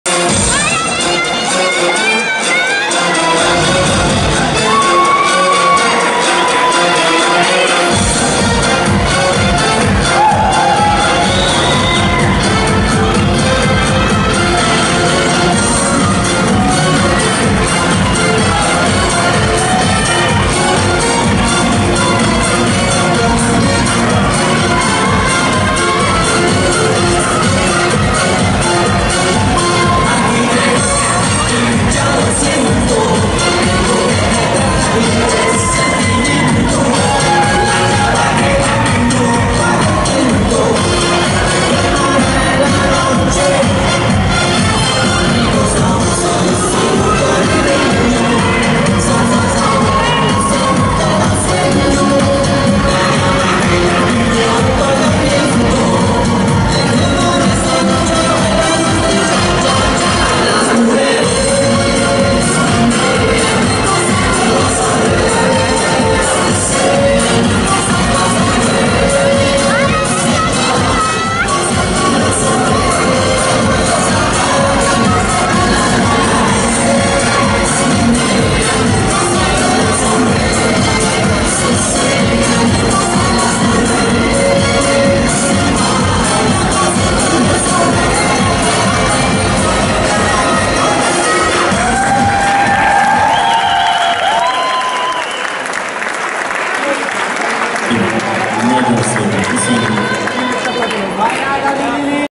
Belucz Levente-Révész Alicia Kistelek Open Jun.I.C. cha-cha-cha
Kistelek Open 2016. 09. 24. Junior I. C. latin 1. helyezett